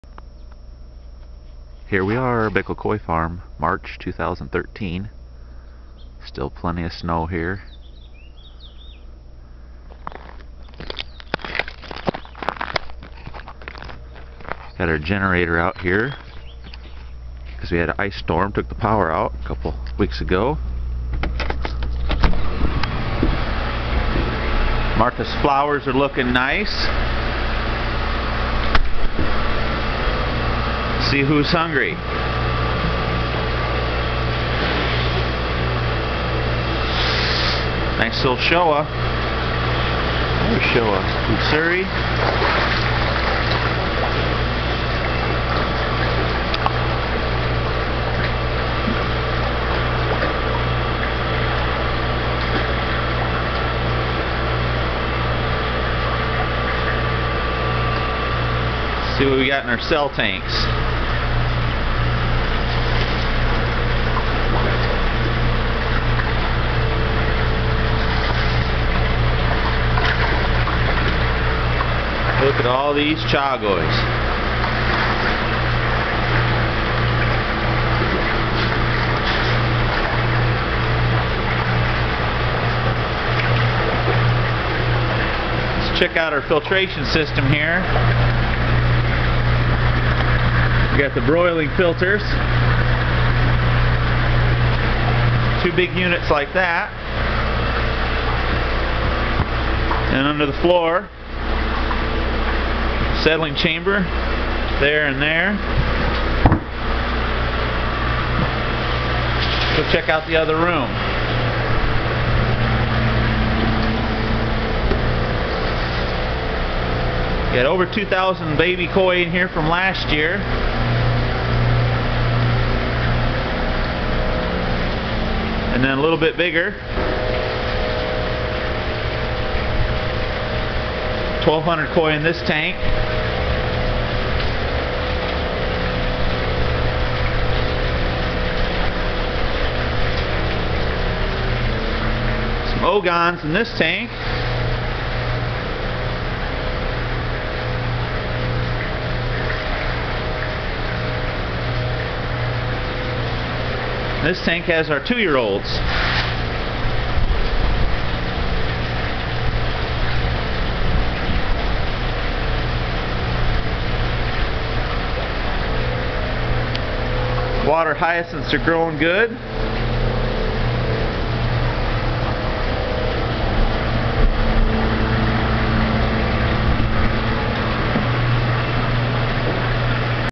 Bickal Koi Farm Koi House
This building is where we keep all of our koi during the winter in Toddville Iowa.  The air in the building is heated which keeps the water warm all winter.  The koi are fed all winter and continue to grow and thrive.  The tanks are all built with cement and lined with rubber.  The filter systems are gravity fed passive settling chambers and then water is pumped to trickle towers and/or moving-bed/bio-reaction chambers.  All of the filtration was designed and build by Greg Bickal.  The roof is constructed of 8mm twin wall polycarbonate panels from FarmTek.